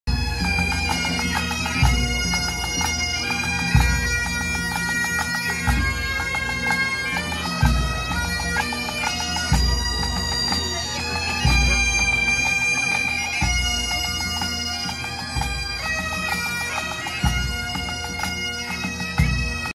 Aubade musicale fête des fleurs 2024 Saint Macaire en Mauges
En prélude du magnifique défilé de fleurs du week end, les musiciens de plusieurs groupes et les danseuses nous ont offert cette belle aubade musicale